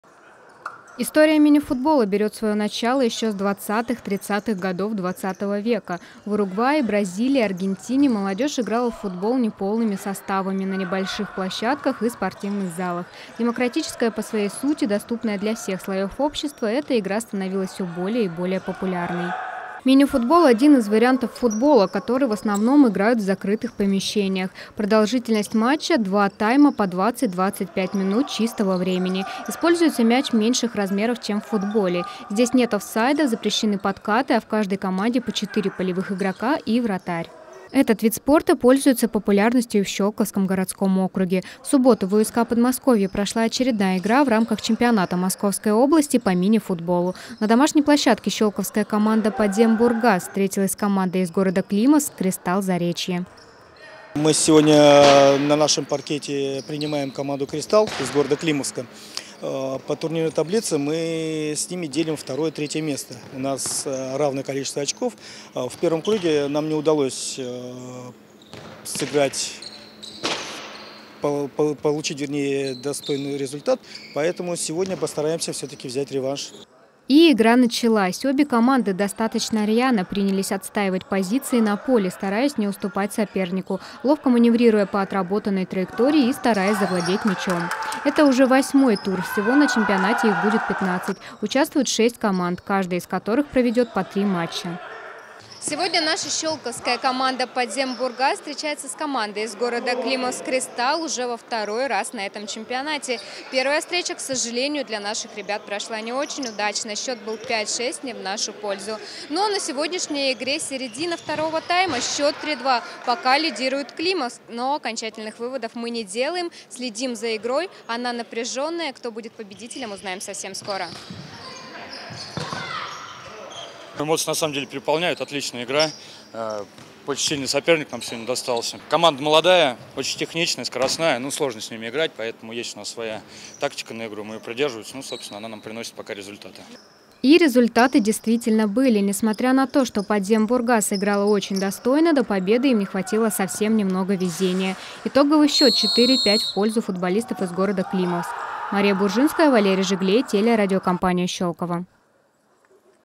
Соревнования регионального масштаба
В универсальном спортивном комплексе «Подмосковье» состоялся очередной матч в рамках чемпионата Московской области по мини-футболу. На поле встретились щёлковская команда «Подзембургаз» и футболисты из города Климовска – команда «Кристалл-Заречье».